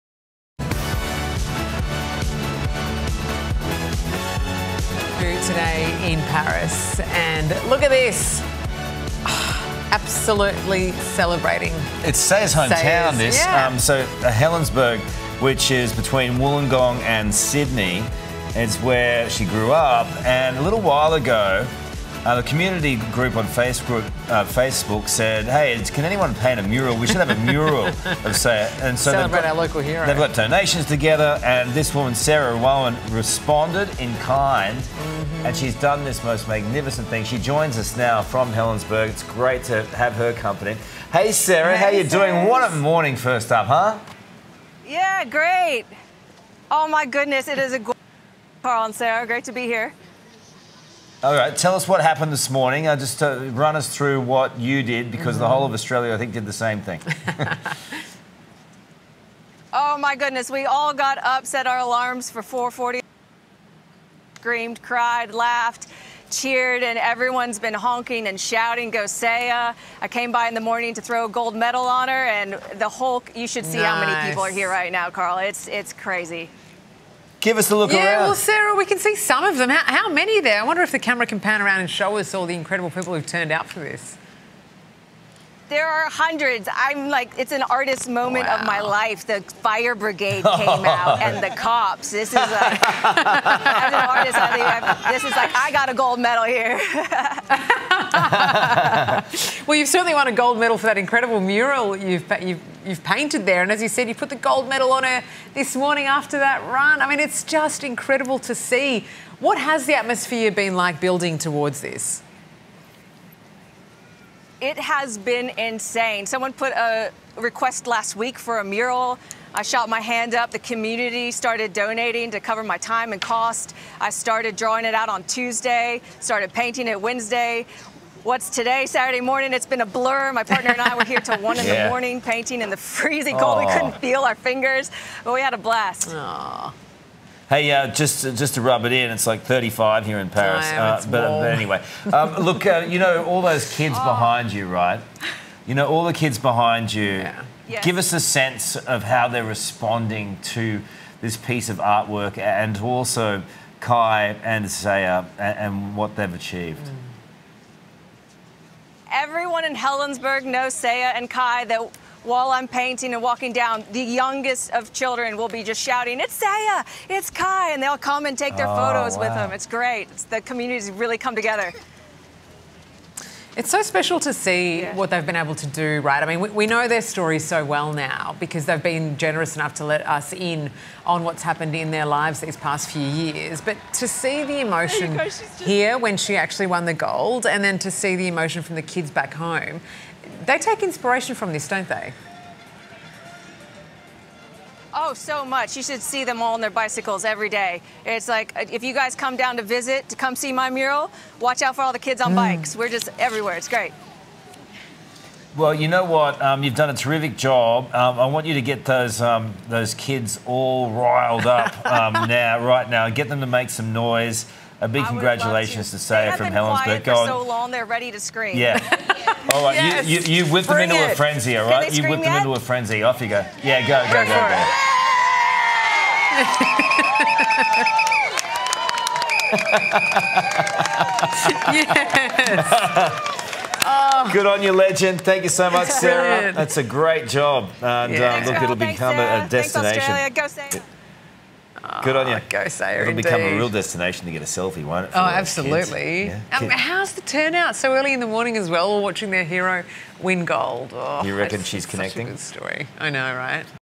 Sarah Rowan + Sakakibara Mural on Olympic News with Channel 9
On August 3, 2024 - Saya Sakakibara became the first medalist for Australia in BMX as she won gold!  Artist Sarah Rowan captured her and her brother Kai on a wall in their hometown of Helensburgh, NSW.

Channel 9 interview with Karl Stefanovic and Sarah Abo LIVE just hours after Saya won gold.